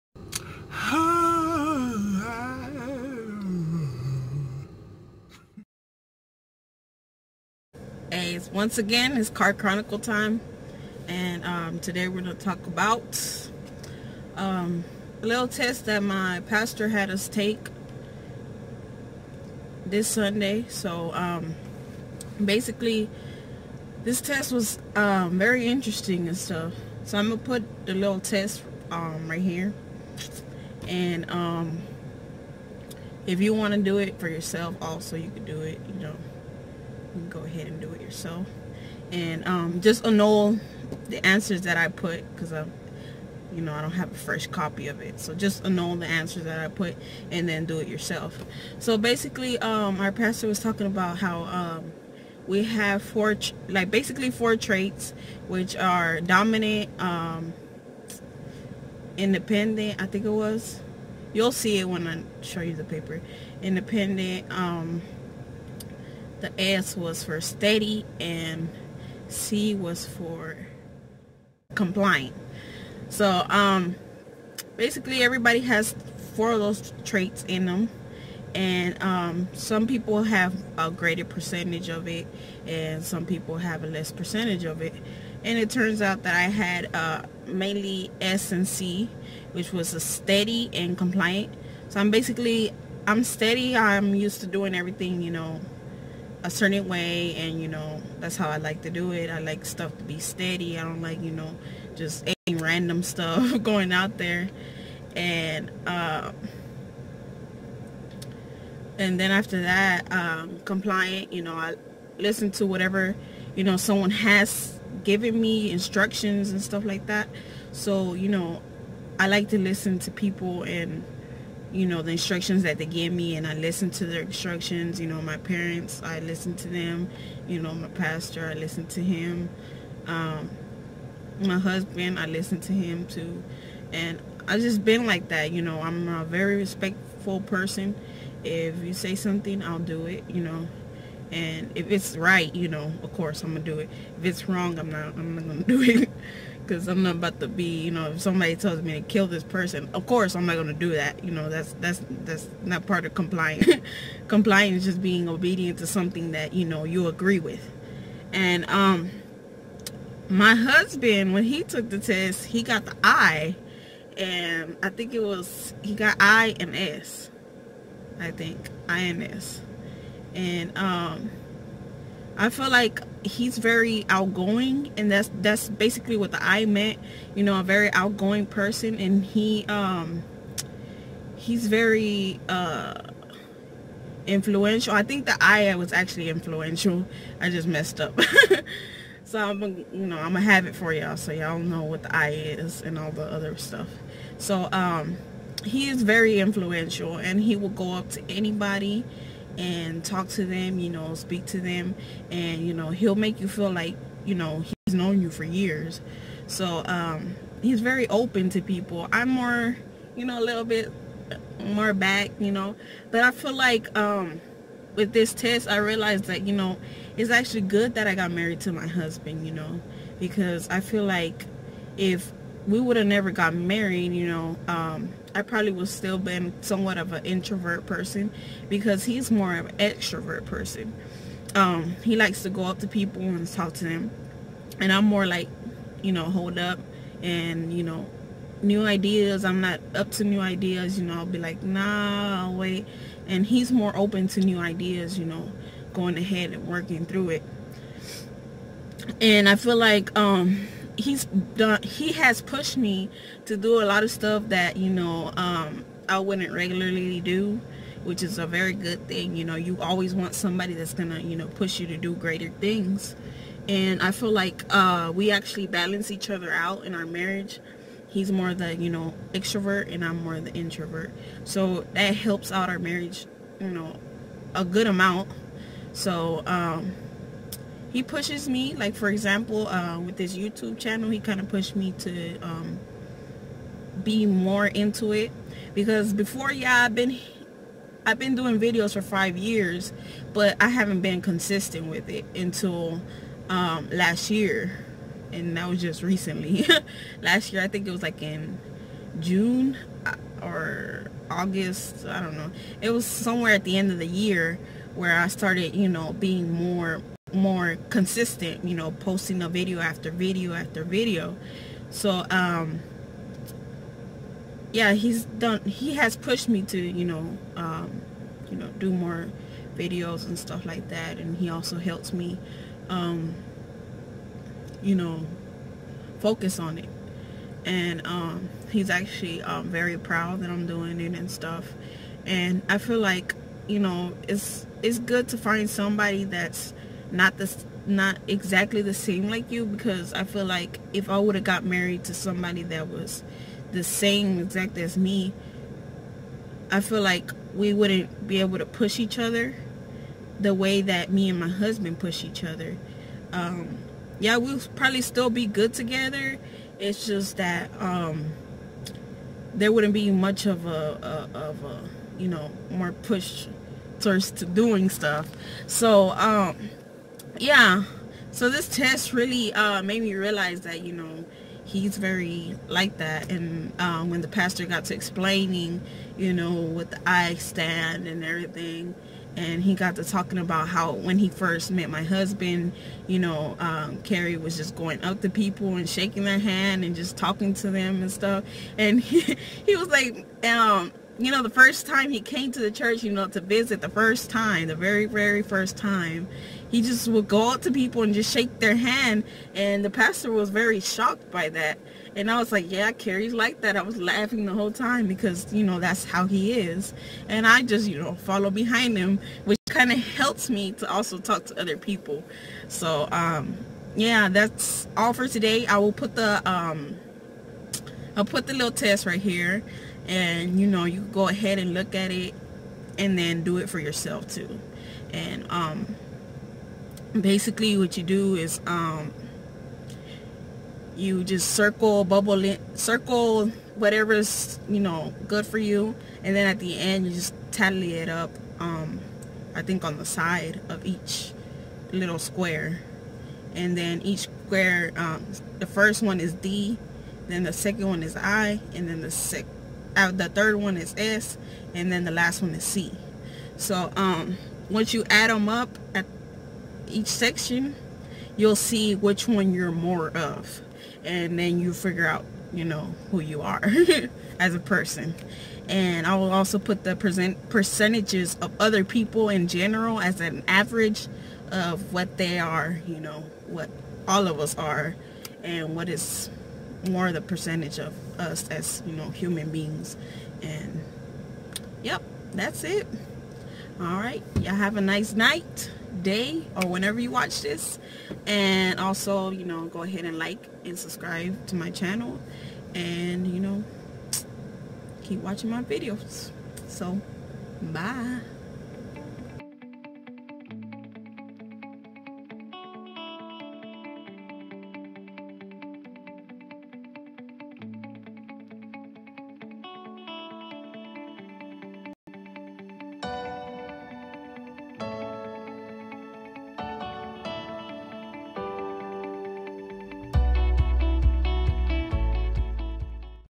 WHO YOU ARE: Discovering your personality
This is a test I just wanna share with yall to discover who you are I really enjoyed taking it and I hope you take the time to take it also.